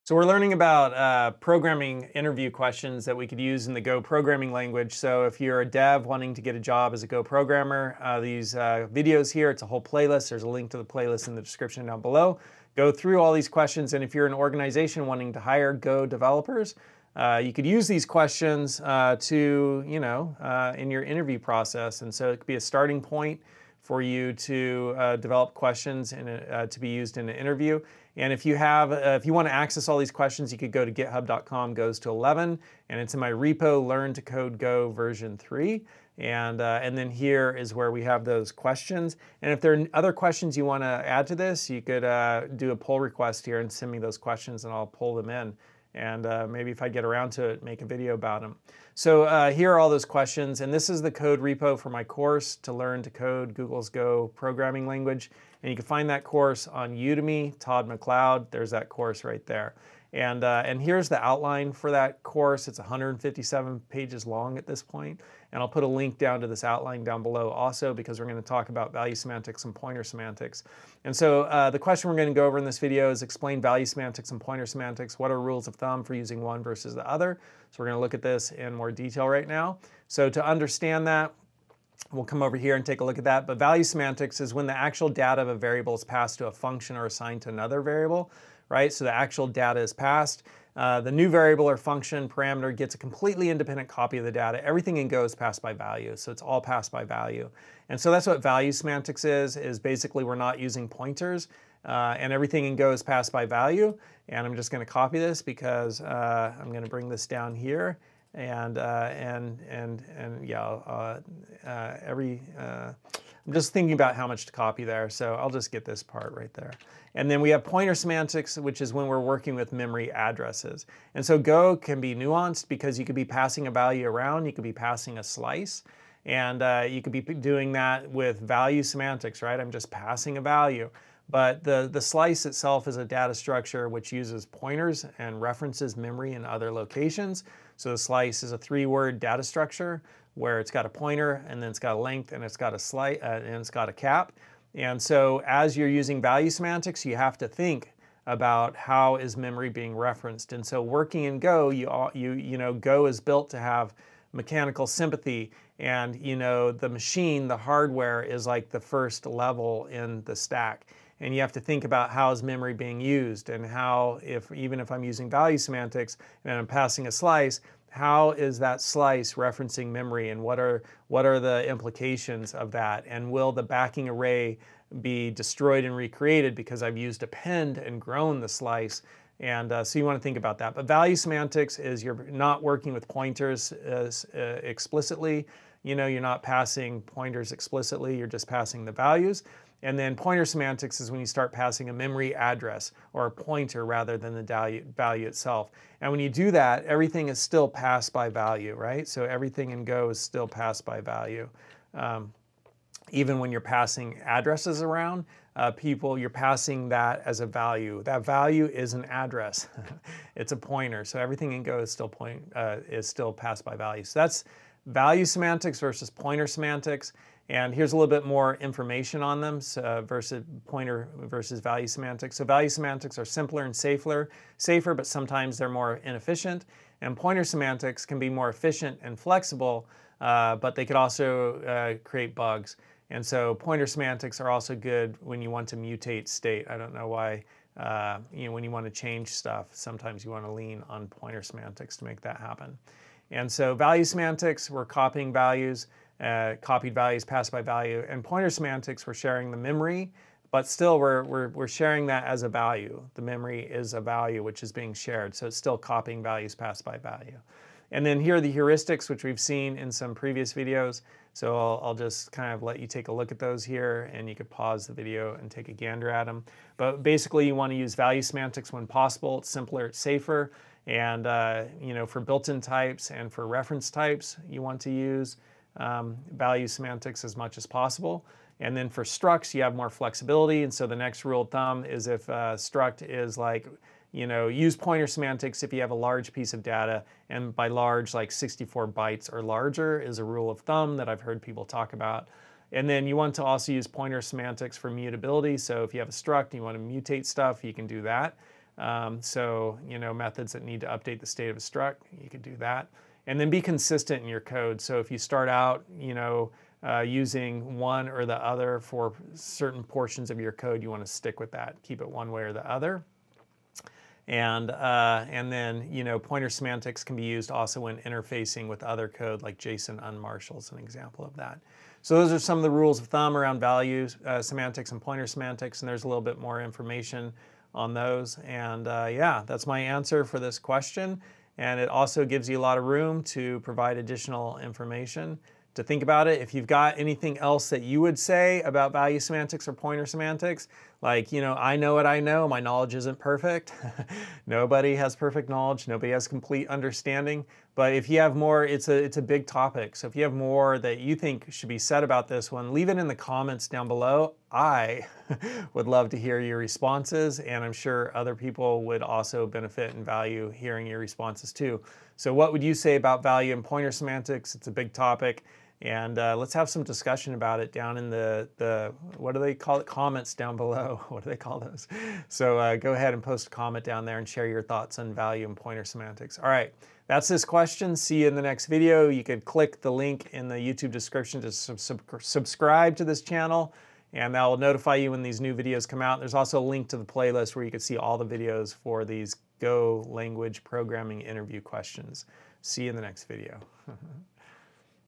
Mastering Go Programming Interview Questions: Value Semantics vs Pointer Semantics
This video provides an introduction to Value Semantics and Pointer Semantics in the Go programming language. It is perfect for developers seeking to land a job in Go programming or organizations looking to hire Go developers. This is your ultimate resource for preparing for your next Go programming interview!